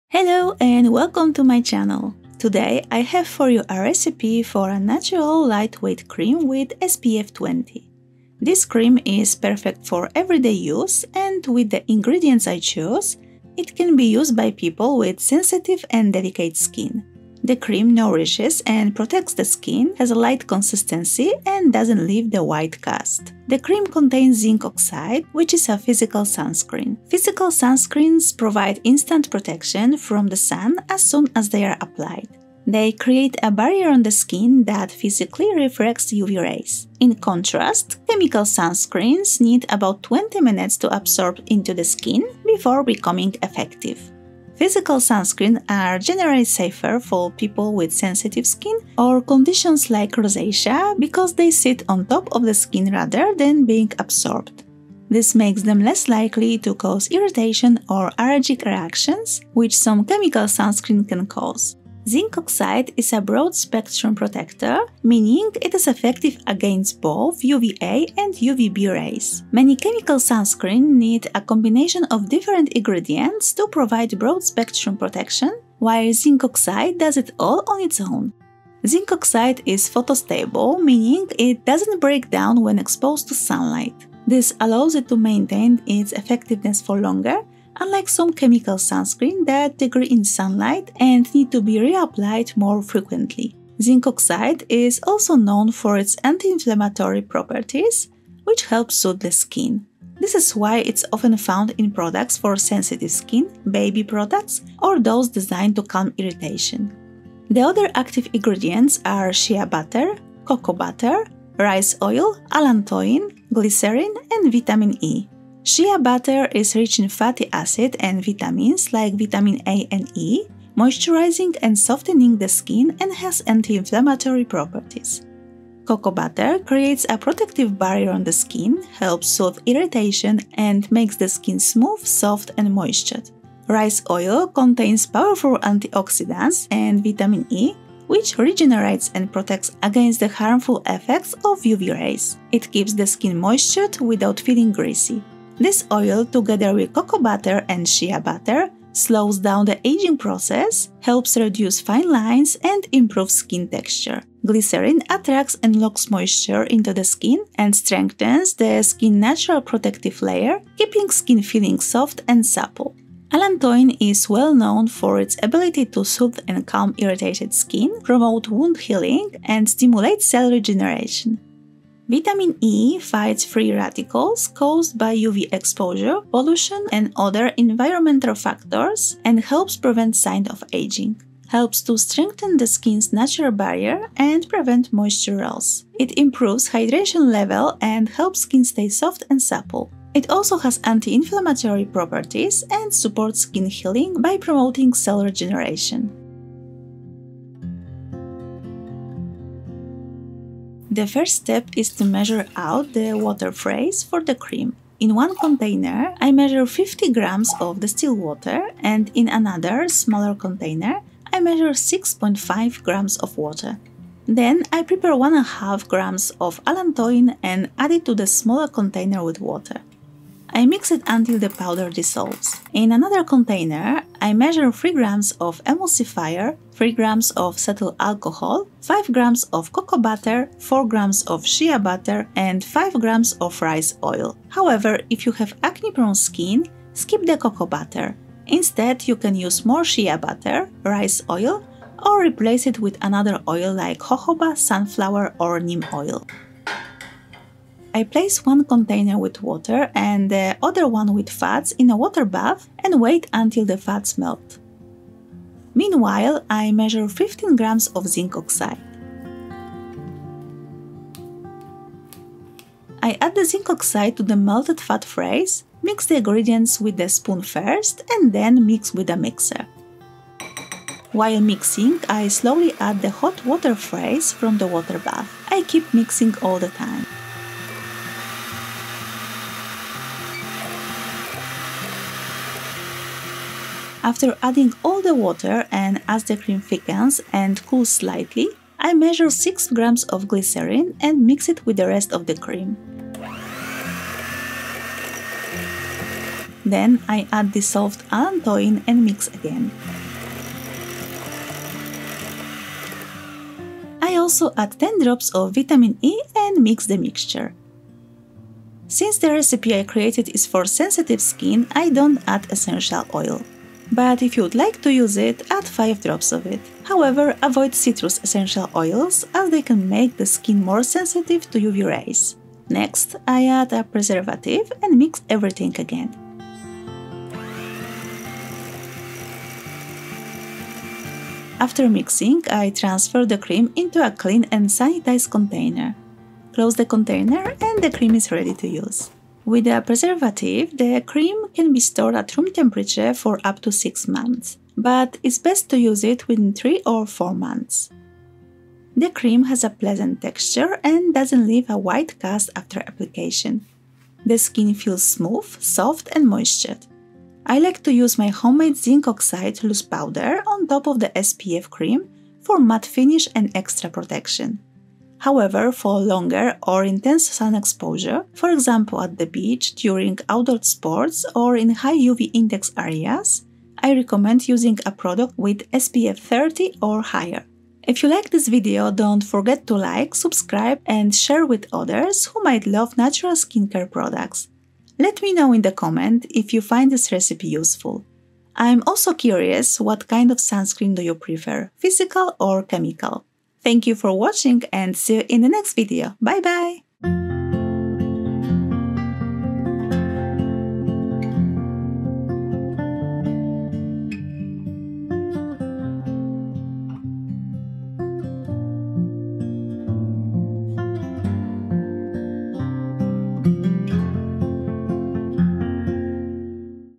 DIY Natural SPF 20 Sunscreen | Soothing Formula with Zinc Oxide & Shea Butter!
Today, I have for you a recipe for a natural, lightweight cream with SPF 20. This cream is perfect for everyday use, and with the ingredients I chose, it can be used by people with sensitive and delicate skin. The cream nourishes and protects the skin, has a light consistency and doesn’t leave a white cast. The cream contains zinc oxide, which is a physical sunscreen. The other active ingredients are: Shea butter, cocoa butter, rice oil, allantoin, glycerin, and vitamin E.

➡️ Shop My Favourite Products Here:

Make Your Own Cosmetics:
Hair Care Products & Accessories:
Beauty Products & Accessories:

INGREDIENTS FOR 100G OF CREAM:
- GSC (Glyceryl Stearate Citrate) emulsifier, 3g
- cetyl alcohol, 3g
- cocoa butter, 5g
- shea butter, 4g
- rice oil, 5g
- zinc oxide, 15g
- distilled water, 56.5g (50g for water bath and 6.5g to dissolve allantoin)
- allantoin, 1.5g
- glycerin, 6g
- preservative, 0.5g - 10 drops (Dehydroacetic Acid, Benzyl Alcohol)
- vitamin E, 0.5g - 10 drops (Tocopherol Acetate)

❗️Always do a patch test before applying any new product to your skin to avoid allergic reactions or irritation. If your skin feels irritated after use, discontinue application.

CHAPTERS:
0:00 Introduction
0:26 Comparison of physical and chemical filters (the effect of zinc oxide on the skin)
1:47 The effect on the skin of other active ingredients in the cream
3:16 Recipe
5:38 Storing the cream
5:48 Application on the skin
6:17 Summary